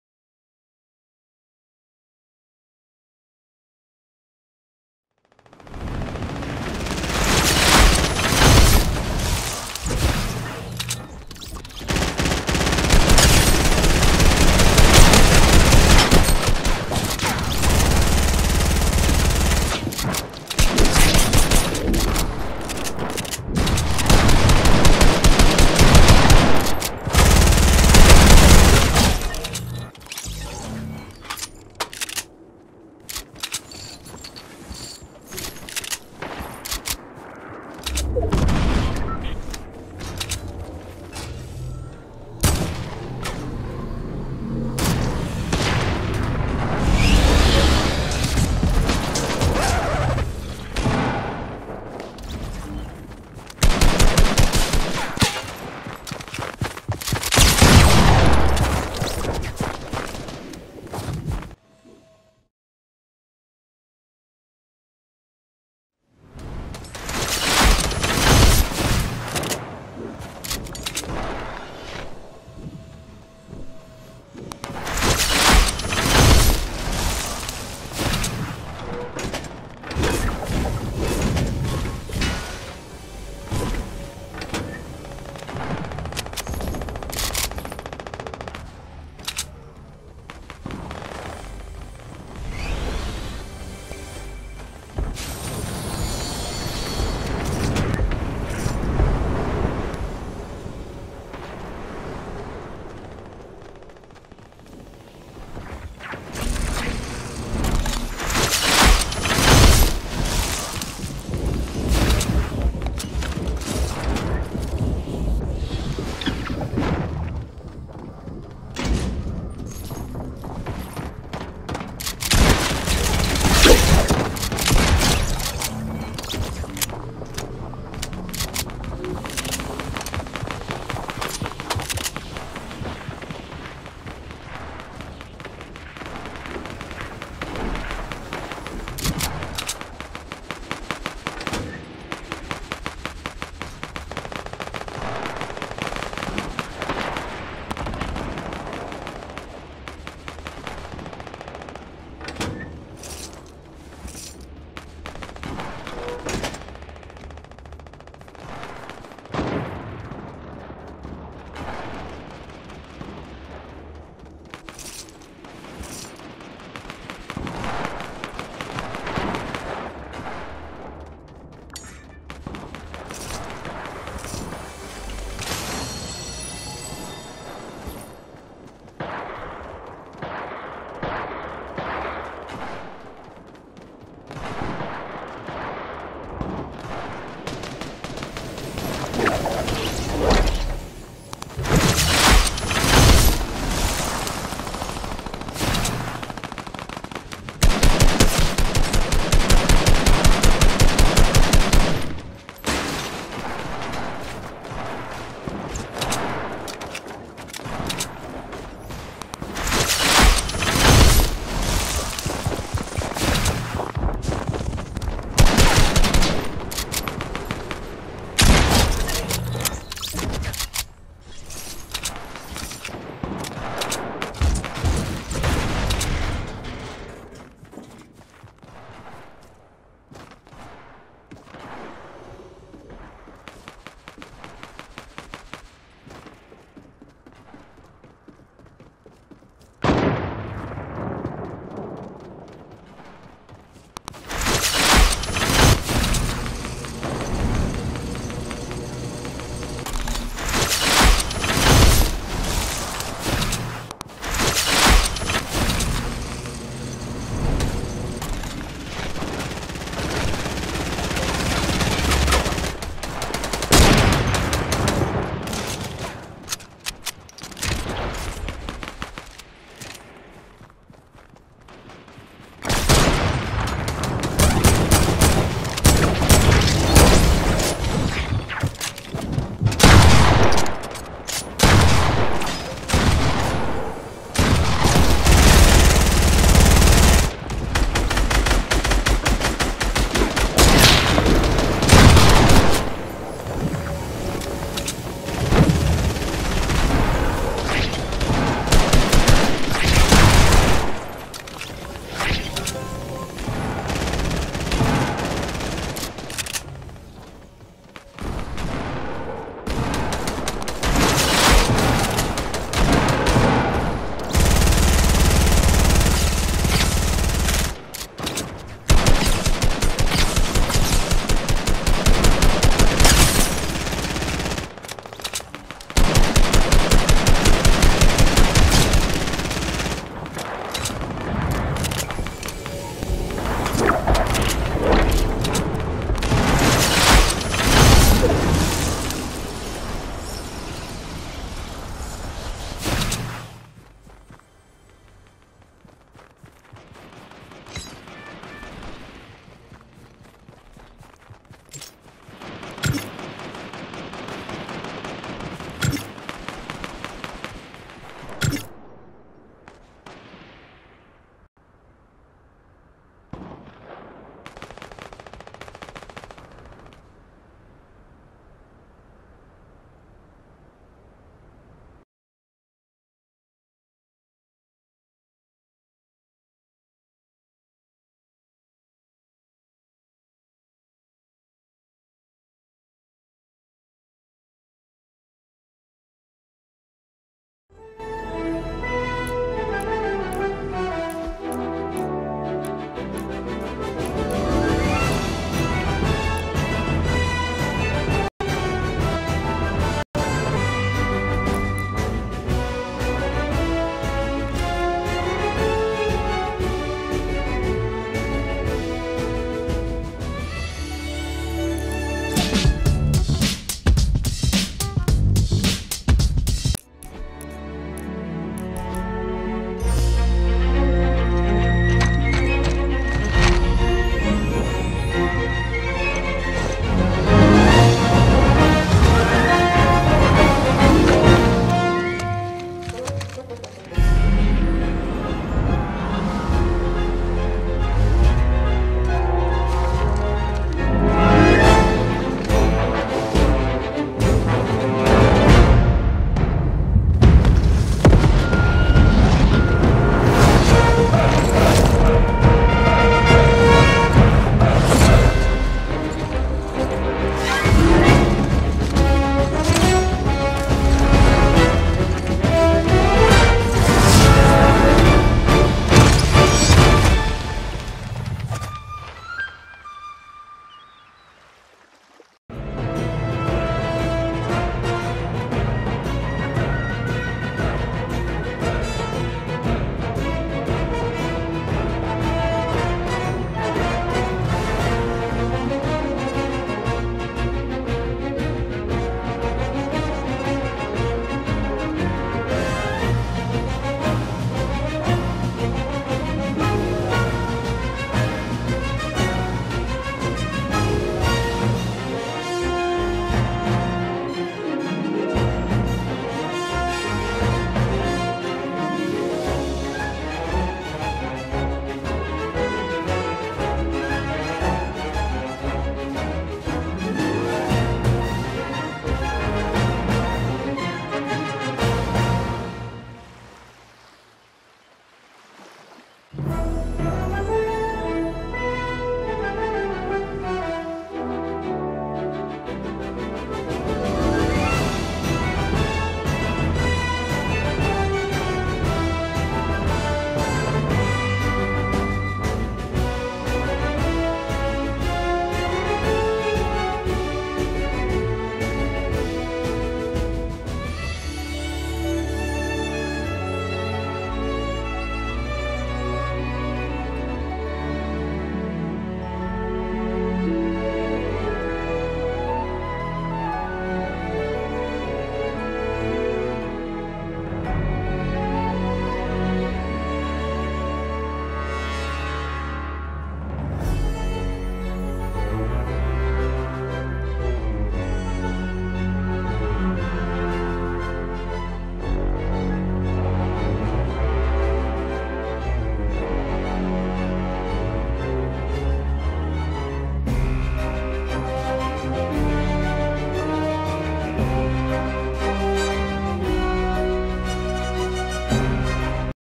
Ragnarok returns to battle noobs part 2
This is the second part of Ragnarok and Carbide returns to battle some really bad players that are bots and are noobs.